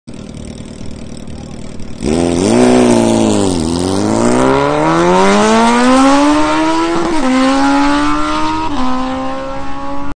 Drag Race 1/4M (402m), 11.08.2018 » Drag Wars SK vs.CZ, automotosprint.cz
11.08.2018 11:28

#219 Suzuki Hayabusa
Kategorie: M4
čas 1/4m: 10.841s
rychlost 1/4m: 213.84km/h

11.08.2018 » Drag Wars SK vs.CZ